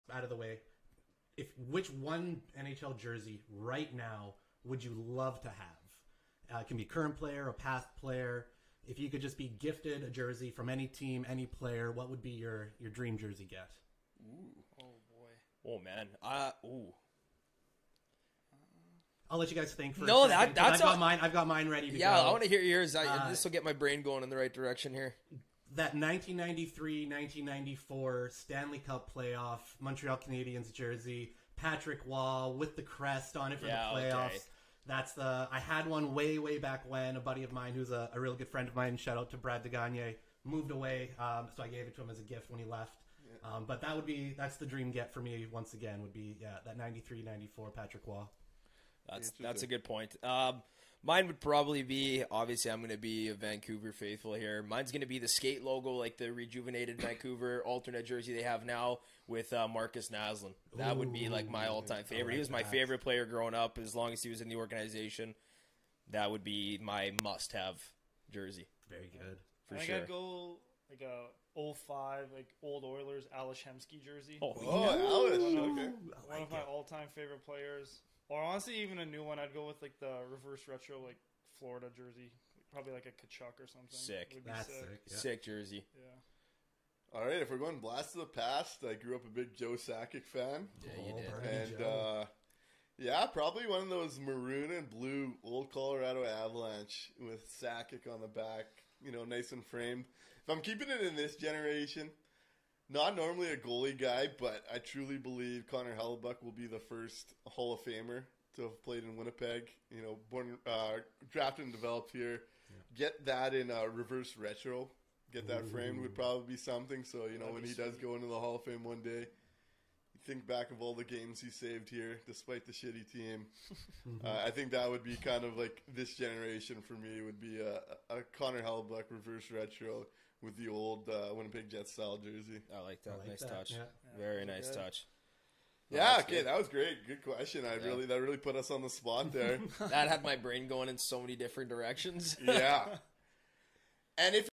The ONE NHL Jersey to Order
On Week 24 TopLyne Black Aces general hockey talk podcast Mikey asks the group which is the ONE NHL Jersey the boys would like to buy if they could pick only one